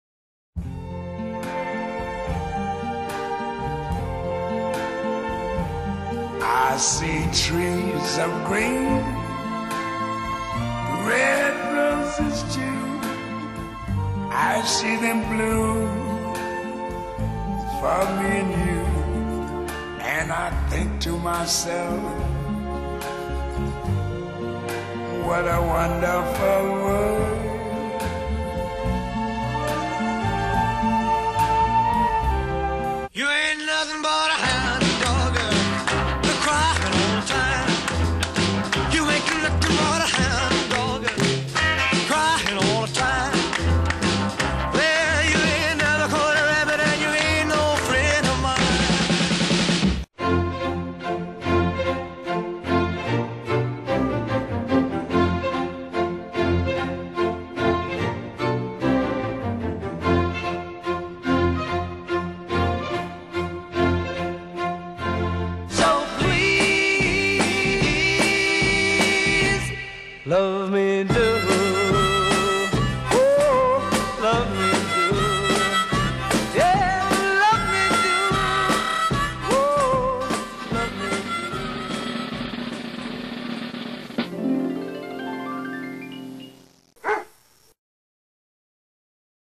Lhasa Apso
Lhasa Apso video produced by EL Minja's kennel , lhasa apso puppies  and at the dogshow with Fun great Lhasa Apso images